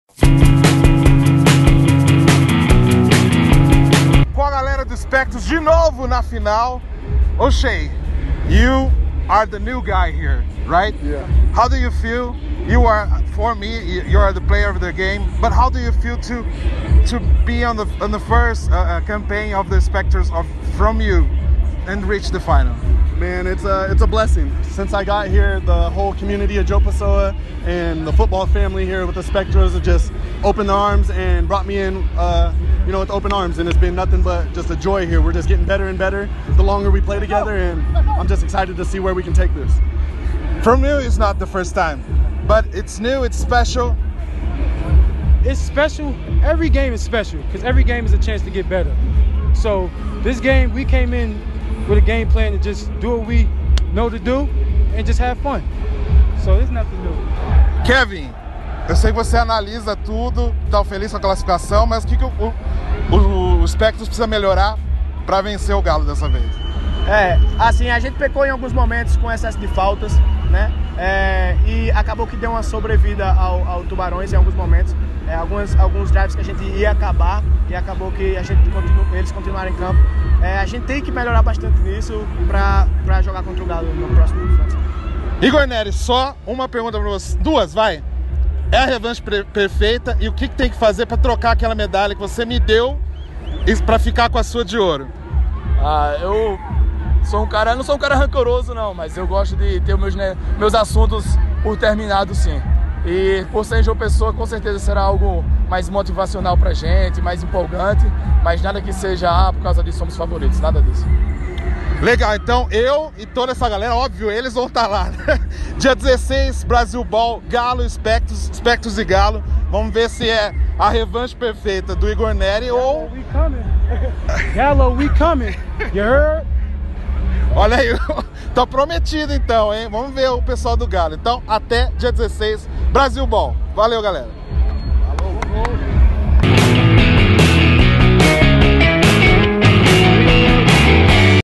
Entrevista Espectros Semifinal BFA 2018 - "We're Coming"
Kevin Veloso, Igor Nery e os americano Oshay Dunmore e Carlos Cox falaram com o Salão Oval sobre a semifinal da BFA 2018 contra o Tubarões do Cerrado e a expectativa de "revanche" contra o Galo FA na Paraíba, no Brasil Bowl IX.